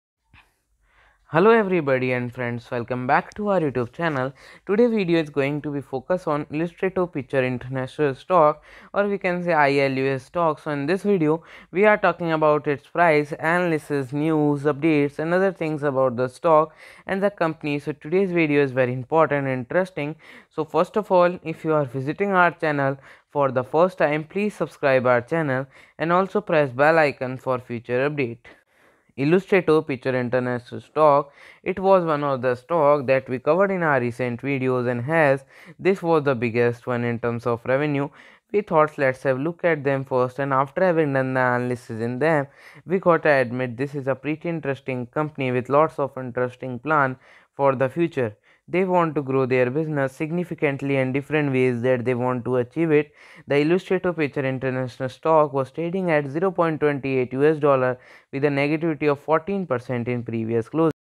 ILUS STOCK - REACTING TO BEARISH GOLD PANDA! BEARISH OR HUGE OPPORTUNITY? ILUS STOCK NEWS TODAY
STOCK REACTING TO BEARISH GOLD PANDA? BEARISH OR HUGE OPPORTUNITY?